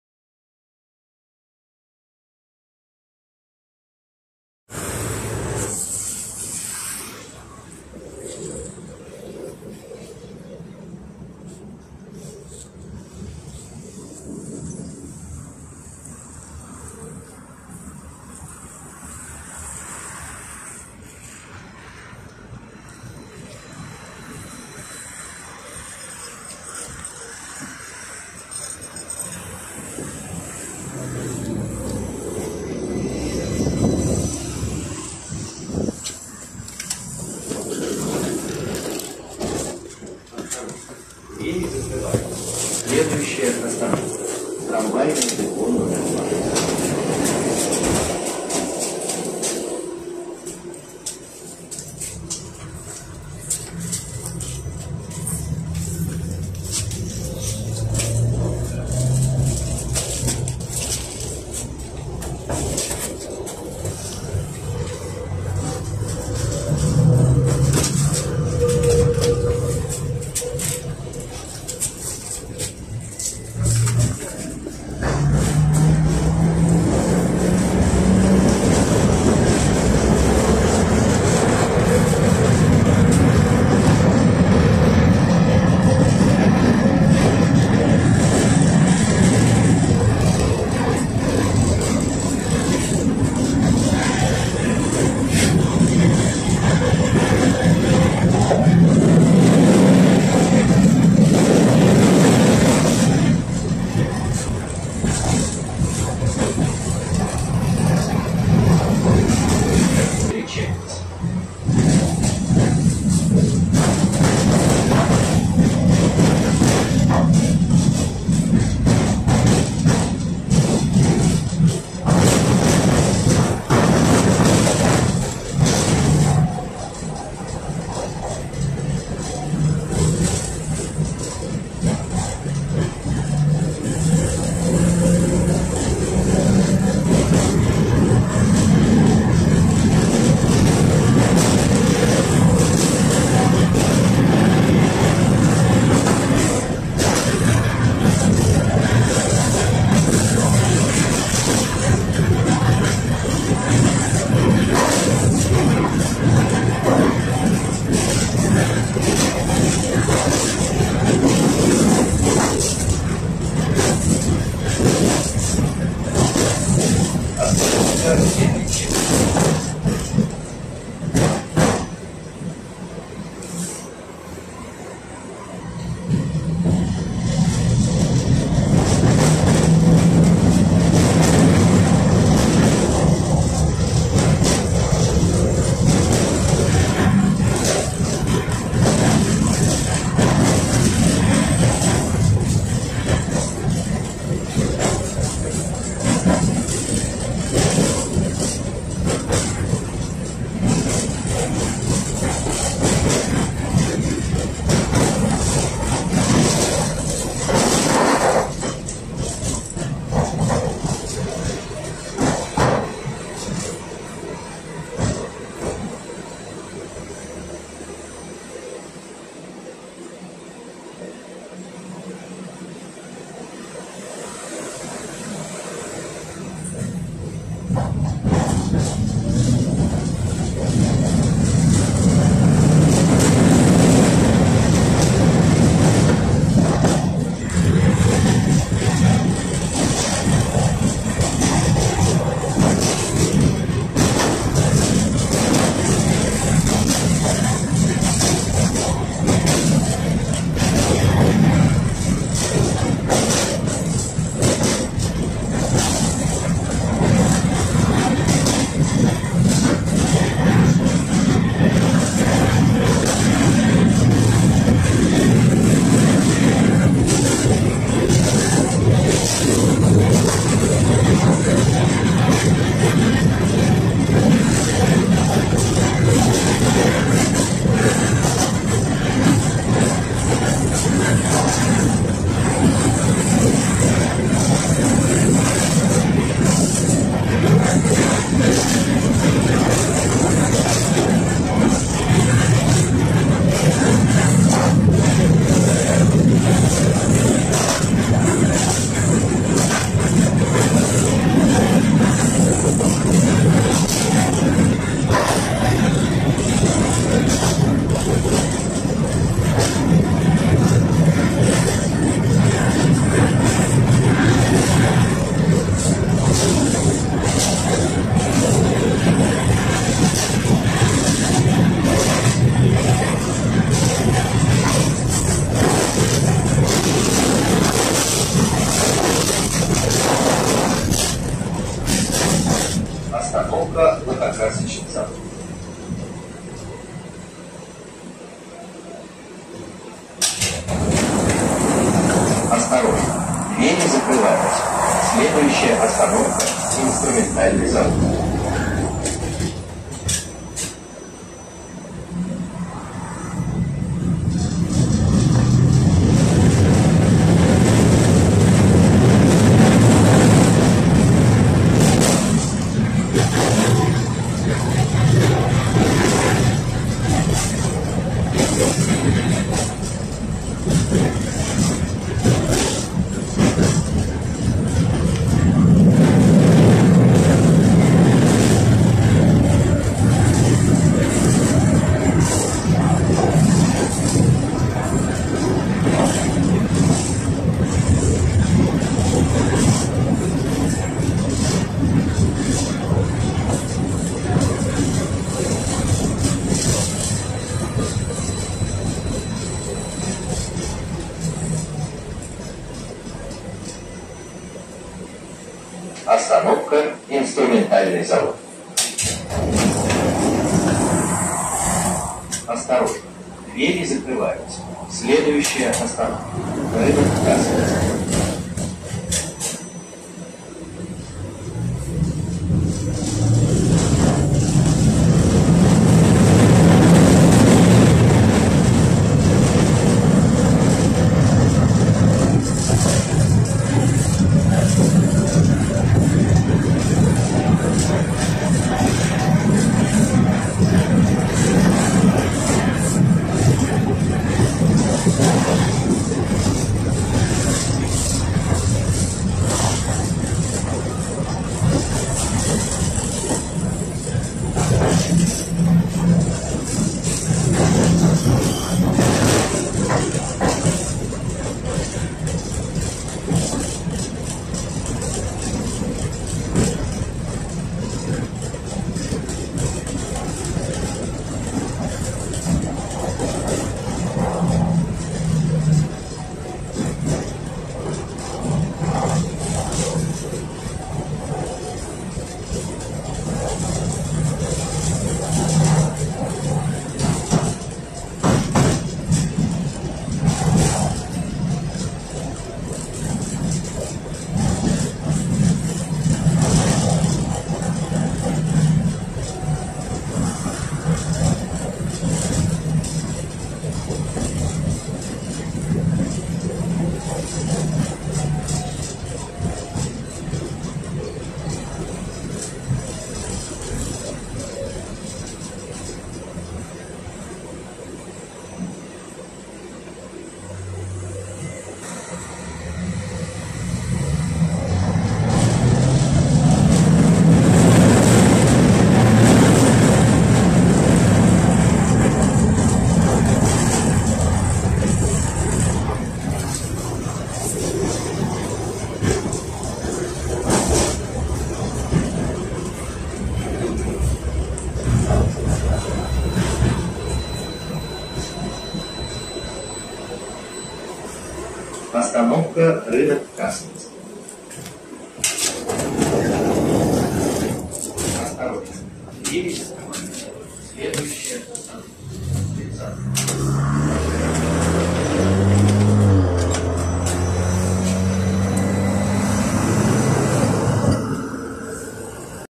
поездка на трамвае КТМ-5М3 ( 71-605 мод Челябинск ), борт 1251, маршрут 18
поездка на трамвае КТМ-5М3 маршрут№18 ЧМК ул. Чистопольская

предприятие ООО челябгэт ( ЧГЭТ )

борт номер 1251
кол-во единиц данной модели ( всего/работают ) 18/16
стоимость проезда 30 рублей по транспортной карте 12.50 по безналу 26 рублей

завод изготовитель Усть-Катавский вагоностроительный завод уквз

двигатель ДК-259Г3
мощность двигателя кВт ???
система управления МПСУ
номинальное напряжение электрического тока тяговых двигателей В ???
кол-во мест общее/для сидения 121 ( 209 макс )/34